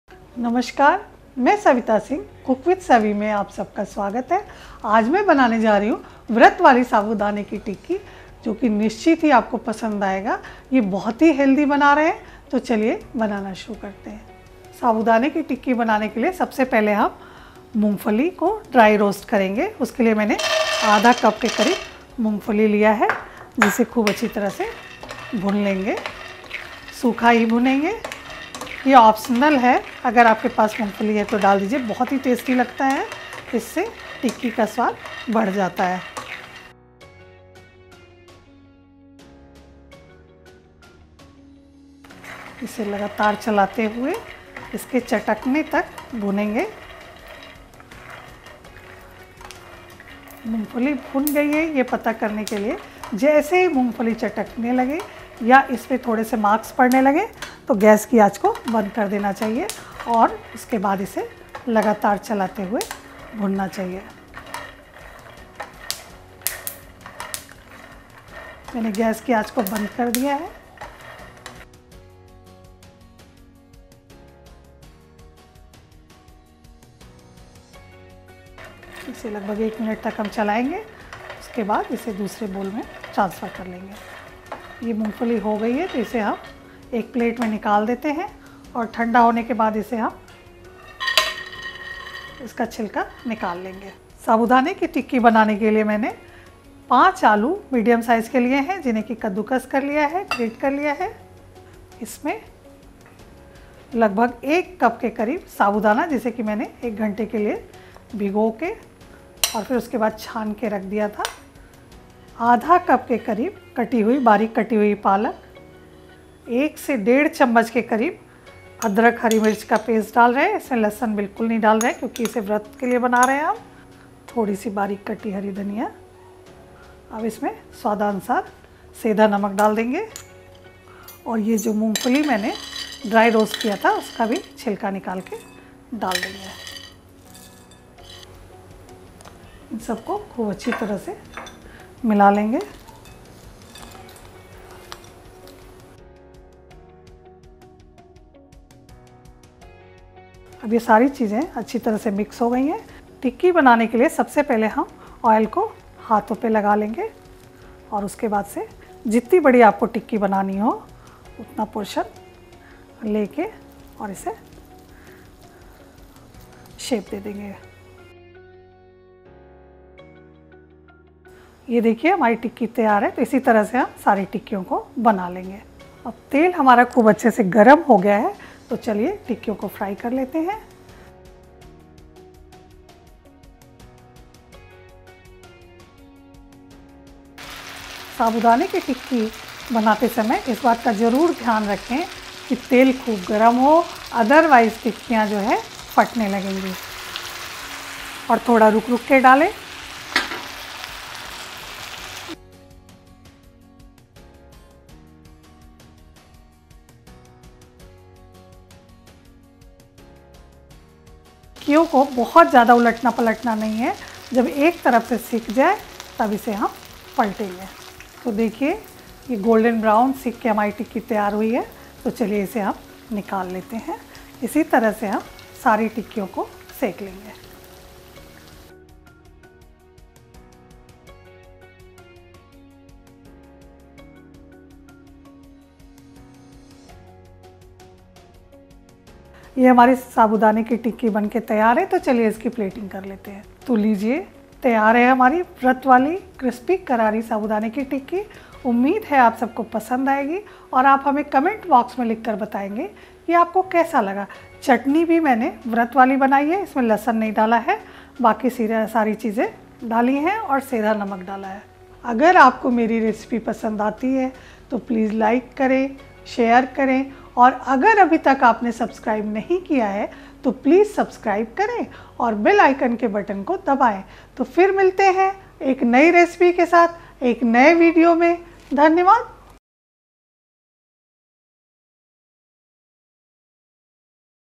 Vrat wali Palak Sabudana Tikki Recipe | साबूदाना आलू टिक्की | Cook with Savi
“Cooking is like painting or writing a song. …”
Learn how to make Crispy Karari Palak Sabudana Tikki Recipe in just a few simple and easy steps